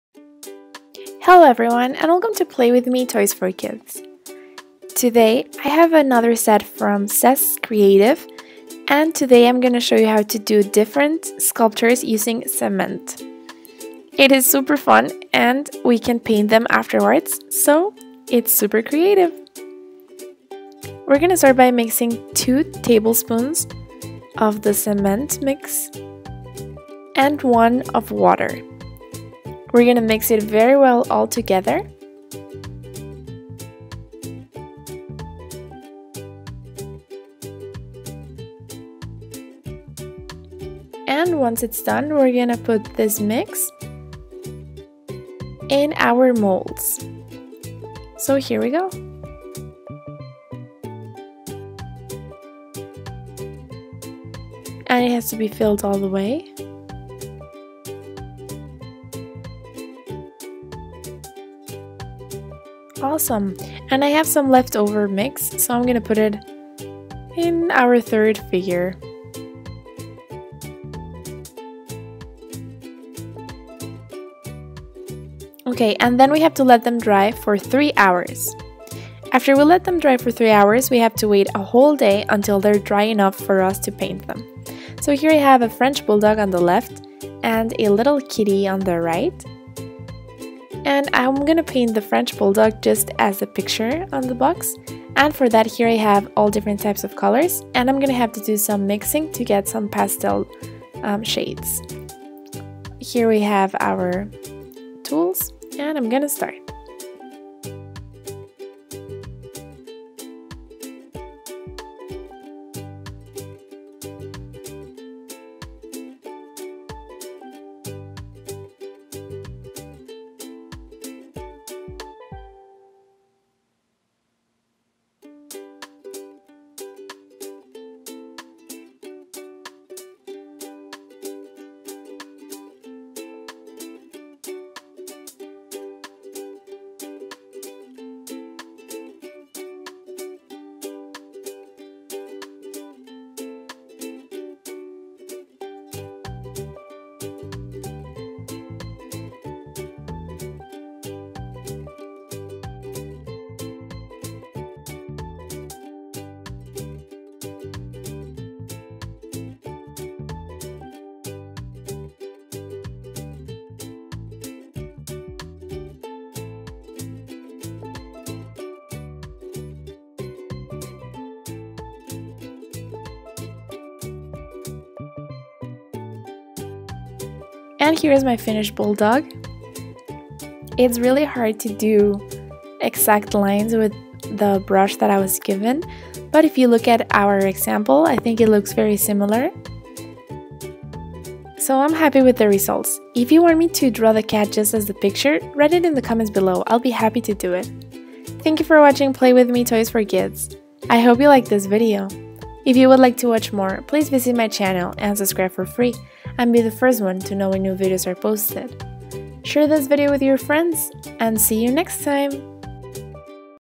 French Bulldog made of cement | Make your own cement figure and paint it! | Painting & Pouring
Lets make arts! Designing a french bulldog with cement

Have fun!